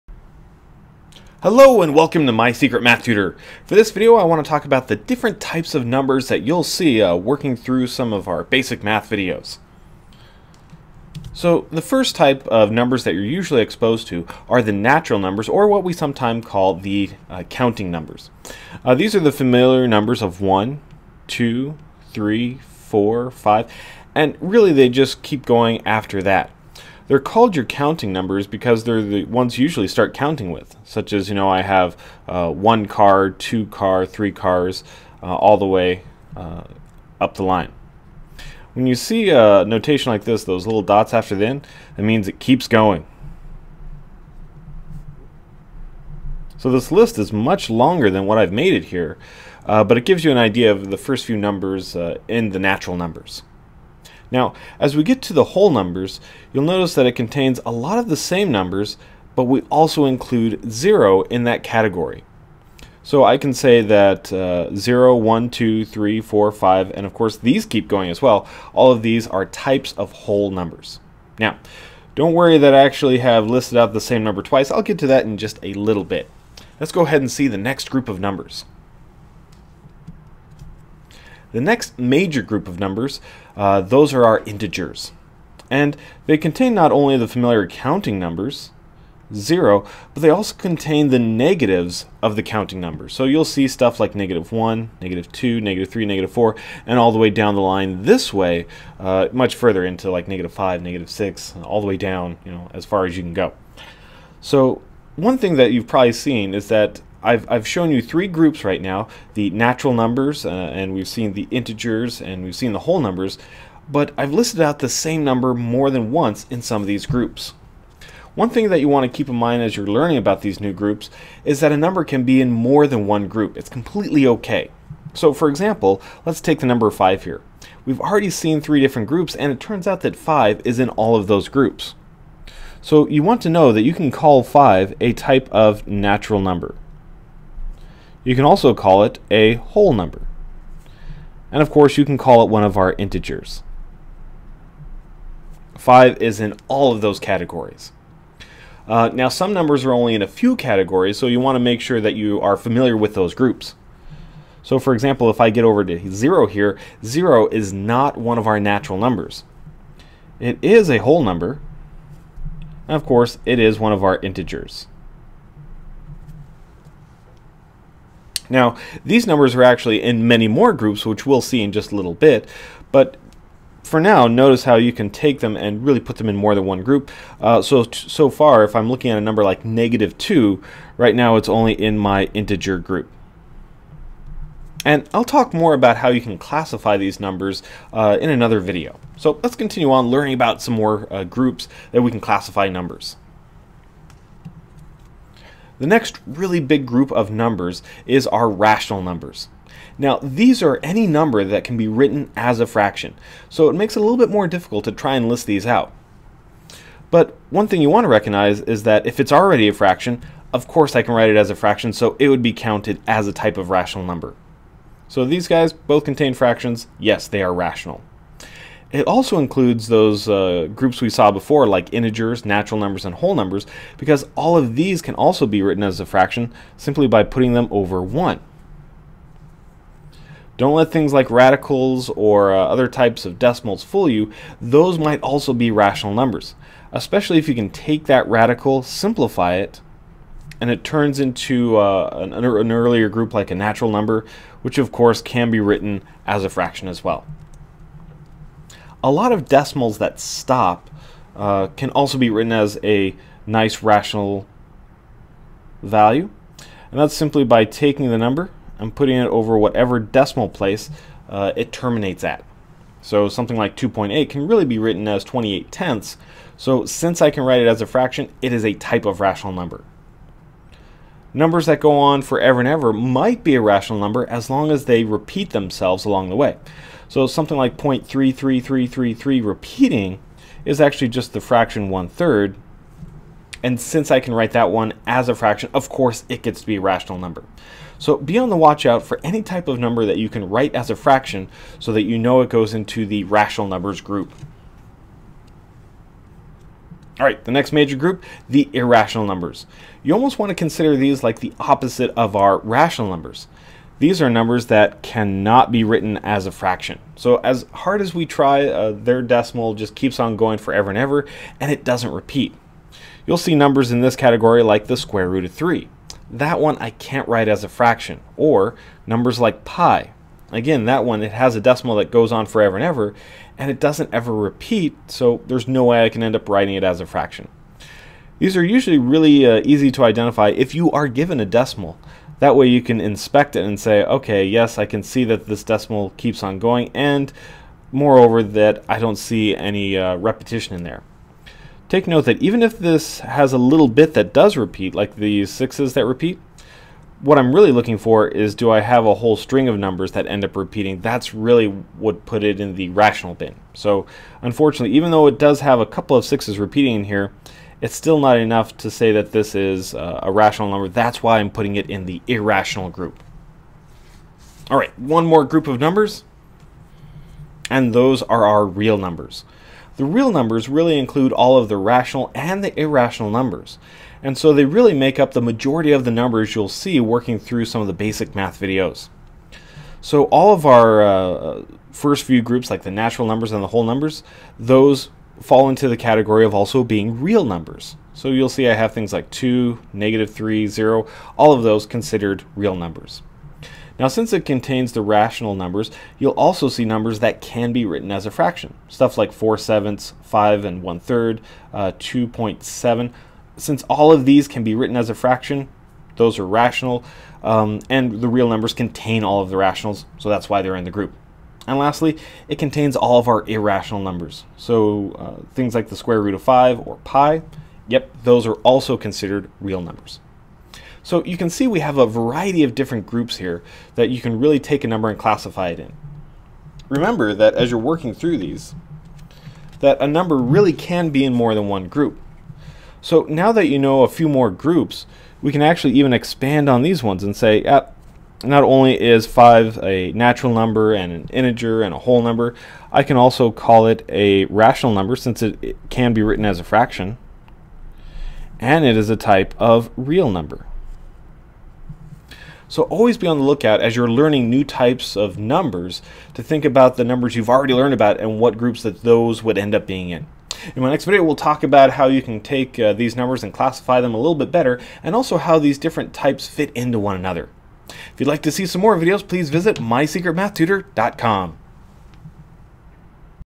Basic Math - Types of numbers
This video covers the major types of numbers that you'll use in math.  These include whole numbers, natural numbers, integers, rational, and irrational numbers.  Pay close attention to which numbers belong in each group.